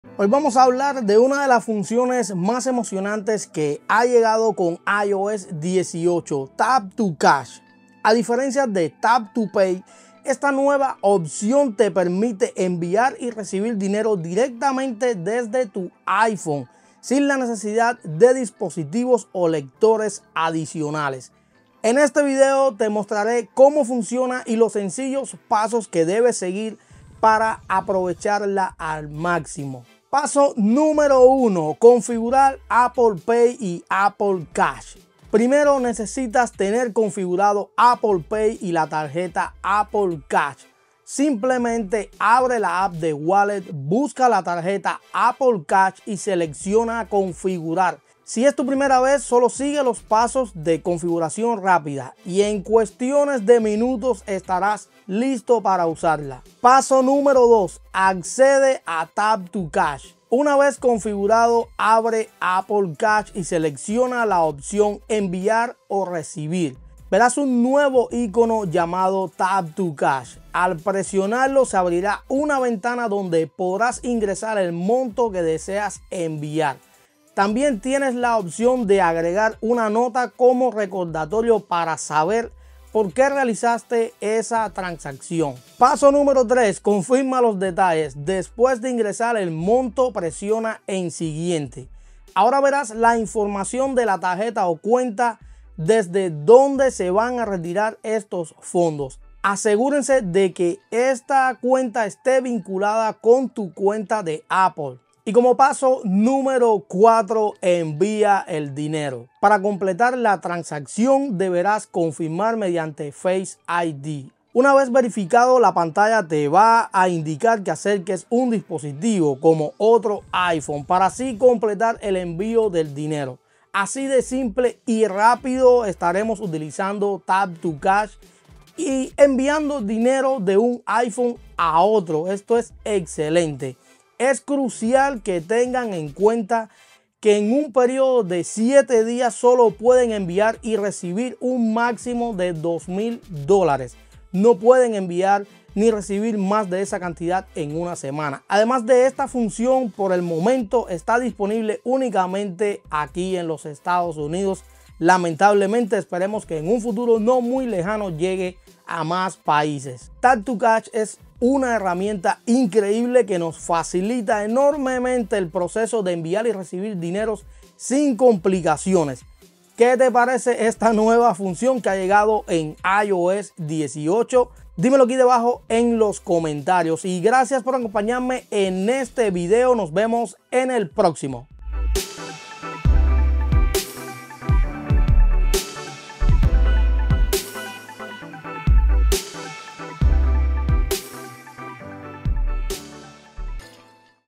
Tap To Cash en iOS 18 Como Funciona
En este video te explico cómo funciona la nueva función Tap to Cash en iOS 18, que te permite enviar y recibir dinero fácilmente sin dispositivos o lectores adicionales. Descubre los pasos que debes seguir para configurarlo y cómo aprovechar esta increíble herramienta en tu iPhone. ¡Mira lo fácil que es!